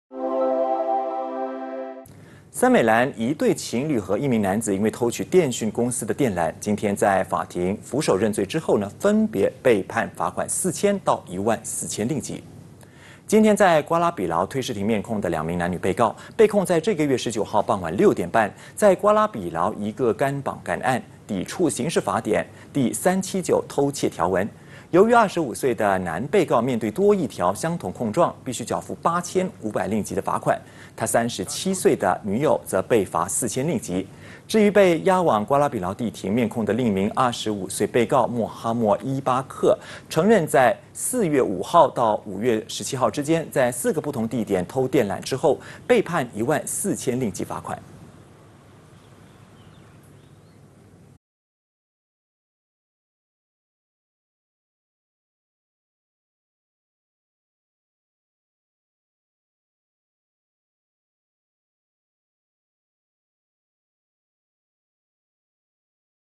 承认偷电讯公司电缆 3人判罚4千至1万4千令吉【2022.06.30 八度空间华语新闻】
【承认偷电讯公司电缆  3人判罚4千至1万4千令吉】
一对情侣和1名男子，在庭上承认偷电讯公司电缆，被判4千令吉或以上的罚款。

【今日主播】叶剑锋 & 陈薇薇